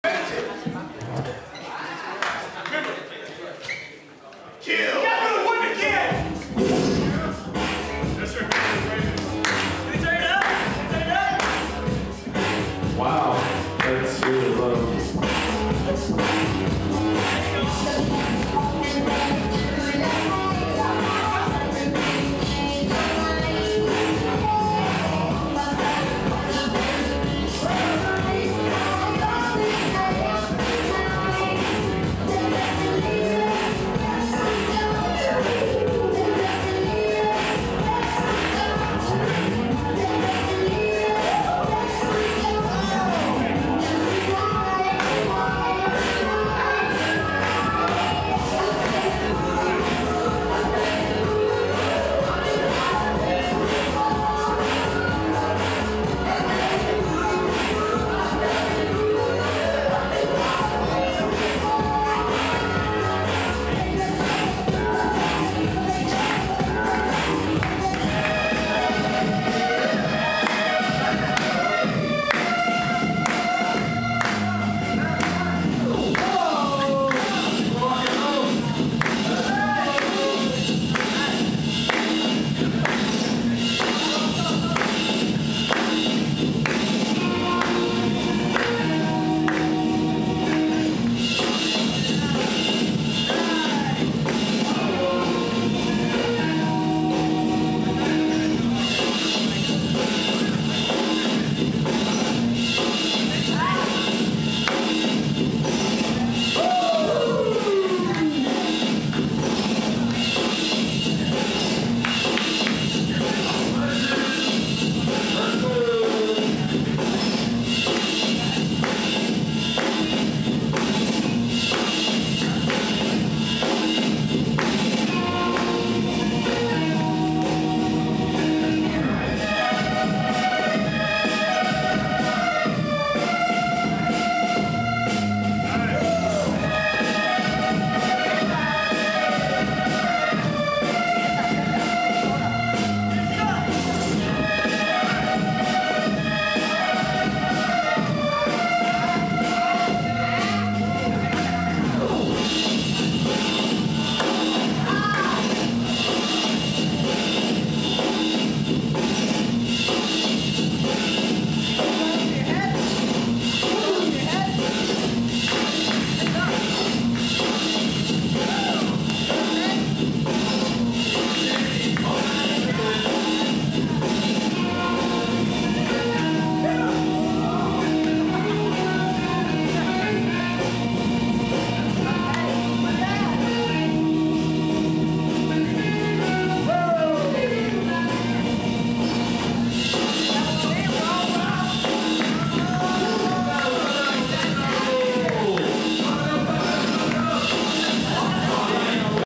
The Ninja & Legend v B-Boy Flow Rock & B-Boy Los
YA Battles on Thursday, November 21, 2013 at The Island, St. Martin's Episcopal Church in Houston, Texas